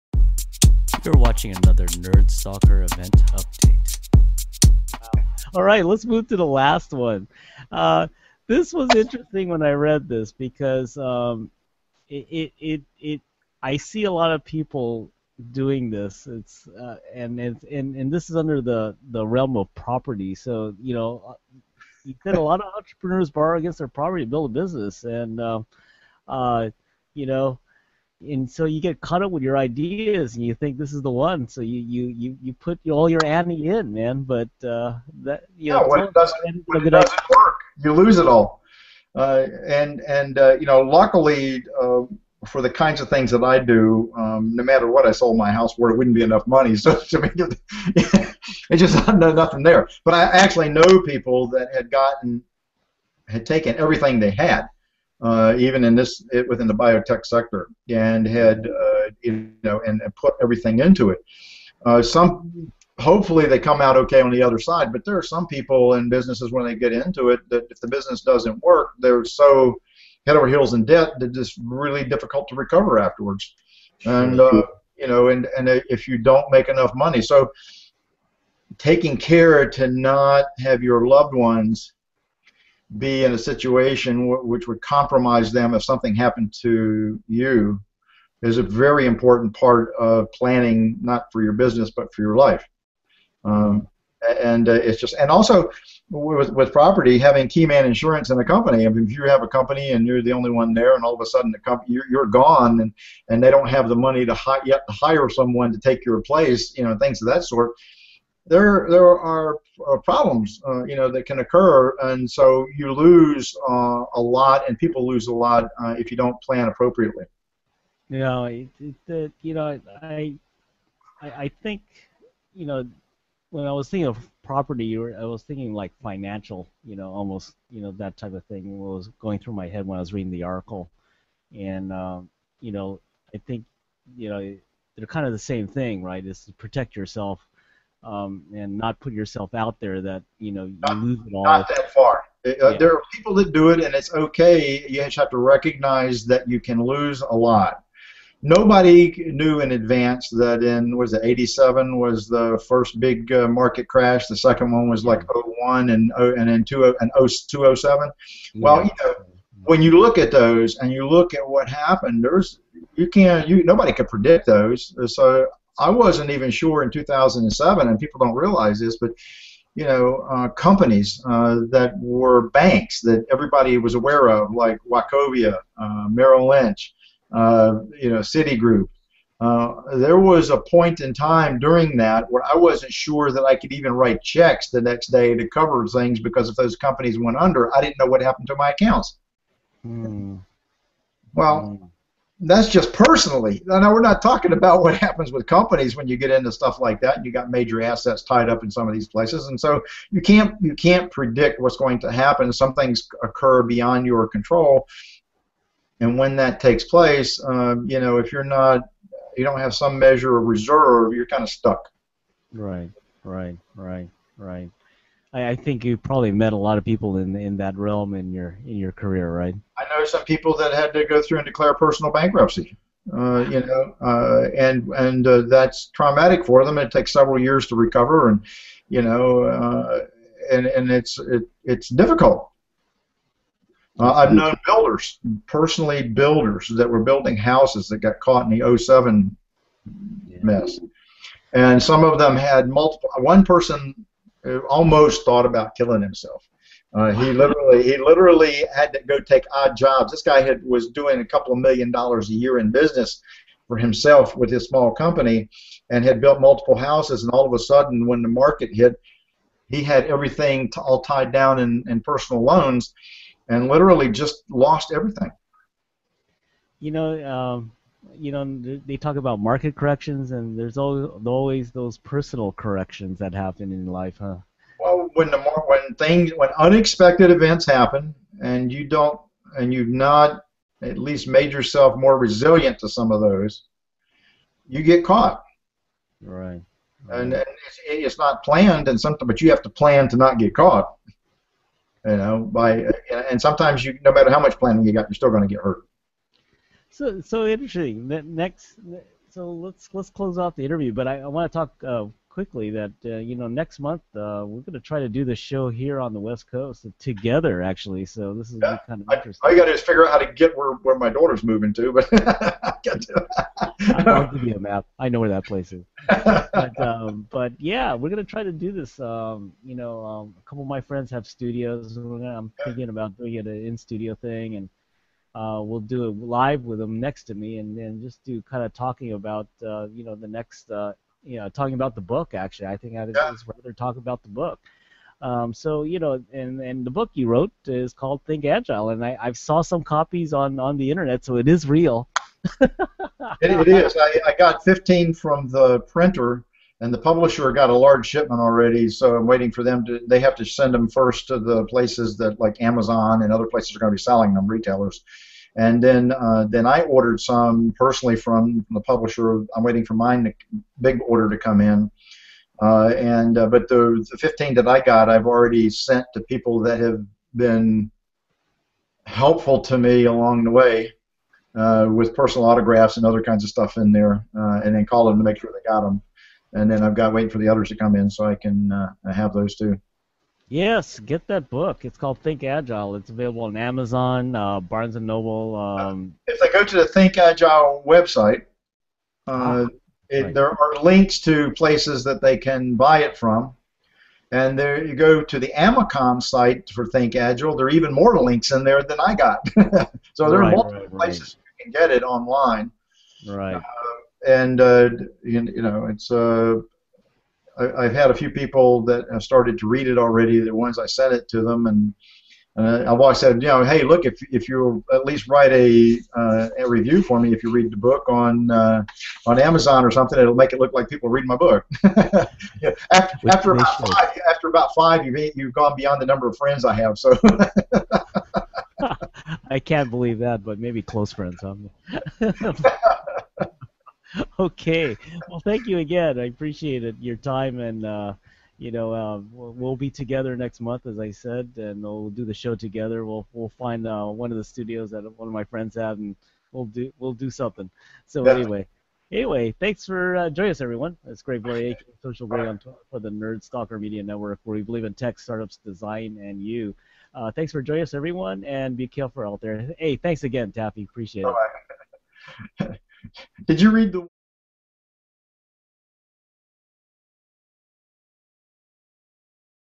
Startup Blog Insights - 5 Fails Agile Entrepreneurs May Regret Part V
We interview Taffy Williams, Examiner.com and entrepreneur, on his examiner article "5 Fails Agile Entrepreneurs May Regret and Never Recover" In this episode, Taffy talks about Property.